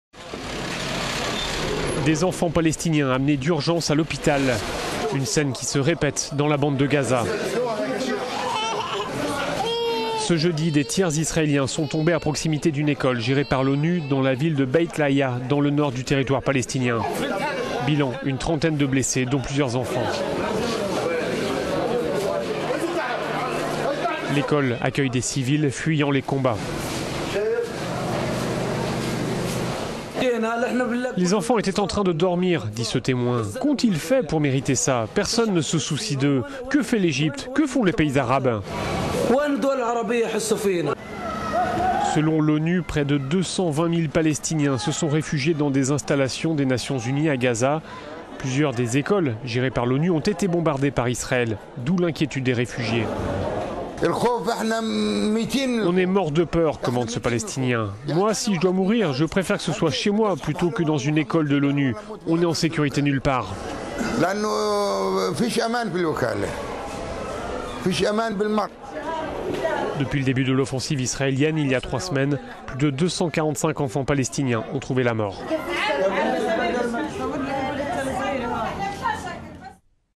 Des enfants de Gaza une nouvelle fois victimes des tirs israéliens
Des enfants palestiniens amenés d'urgence à l'hôpital : une scène qui se répète dans la bande de Gaza. Ce jeudi, des tirs israéliens sont tombés à proximité d'une école gérée par l'ONU dans la ville de Beit Lahiya, dans le nord du territoire palestinien. Bilan : une trentaine de blessées, dont plusieurs enfants.

L'école où ils se trouvaient accueille des civils fuyant les combats. "Les enfants étaient en train de dormir, dit ce témoin. Qu'ont-ils fait pour mériter ça ? Personne ne soucie d'eux.…